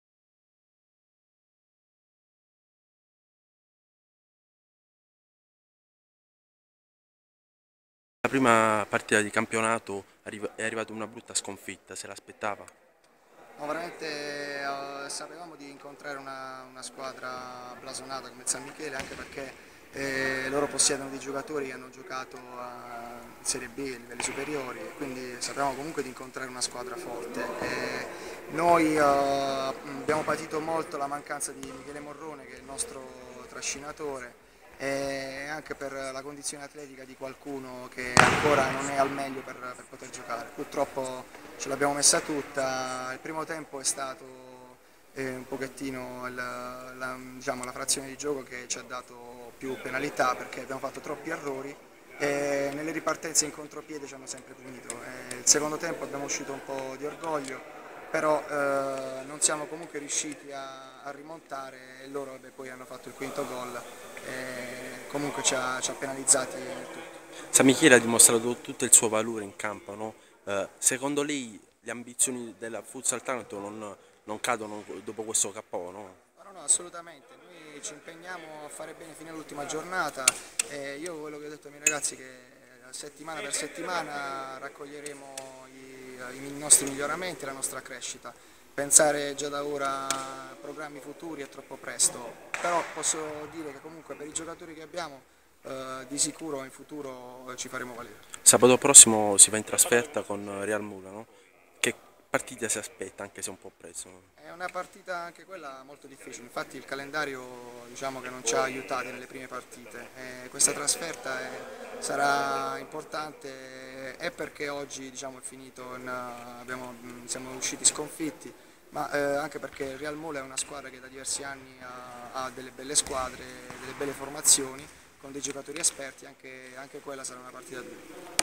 FUTSAL TARANTO - SAMMICHELE 1-5... L'intervista a Dino Guarino (Resp. Tecnico Futsal Taranto)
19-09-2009: FUTSAL TARANTO - SAMMICHELE 1-5... L'intervista  a Dino Guarino (Resp.Tecnico Futsal Taranto)